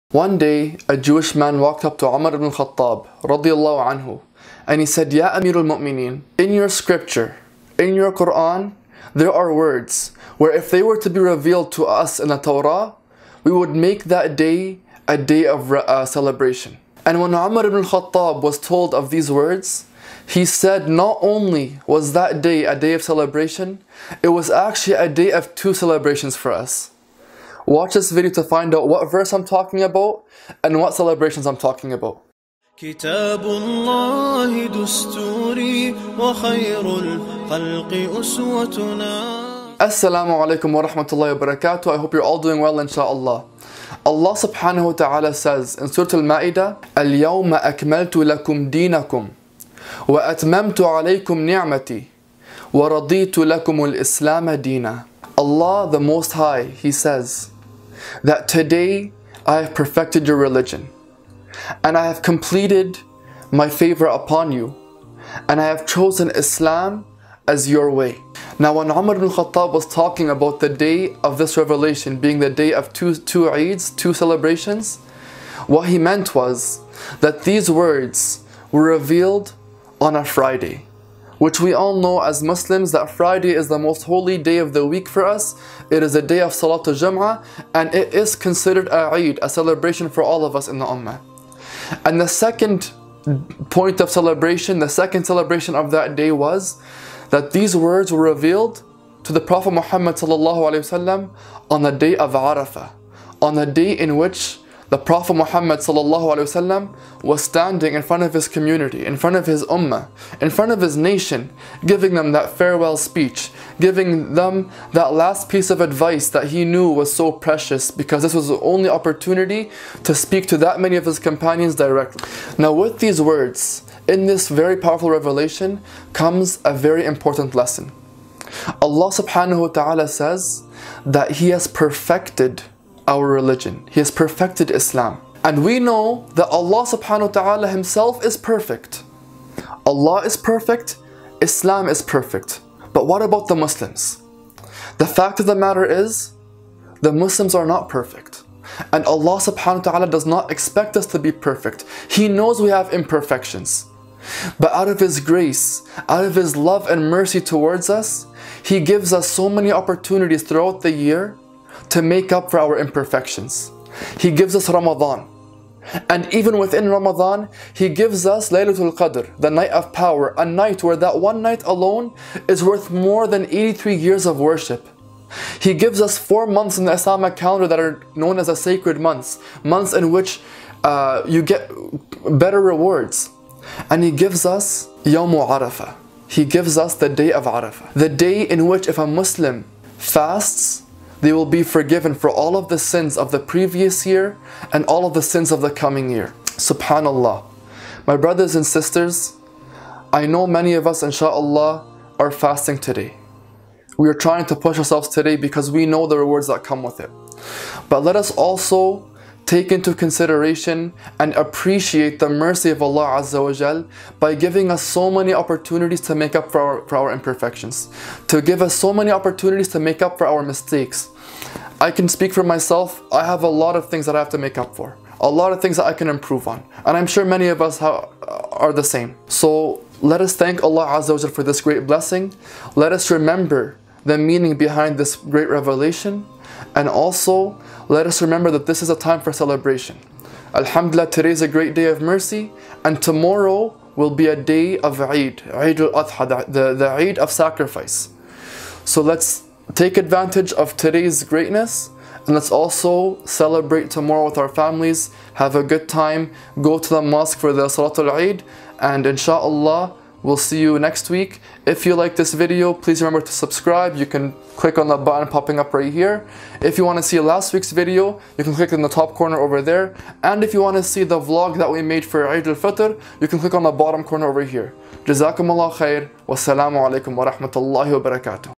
Islam is Perfect, but What About the Muslims?
An Important lesson from the day of Arafah, we as Muslims follow a perfect religion, a perfect way of life, but are we as Muslims perfect? No. No one is perfect, but it is because of Gods mercy that we have so many opportunities to make up for our imperfections.